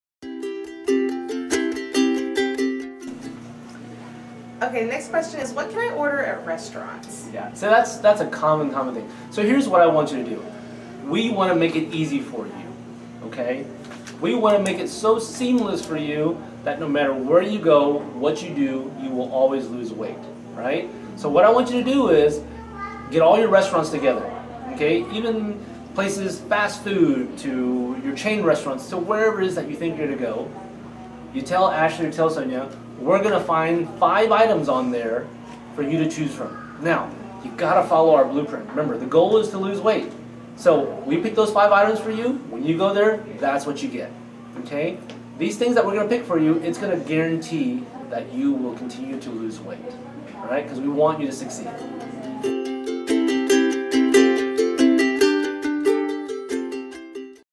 Now that I've Started the Diet, What Kind of Food Can I Still Eat At Restaurants?
We know that you will want to eat out from time to time so Dr. Thai is here to explain your options.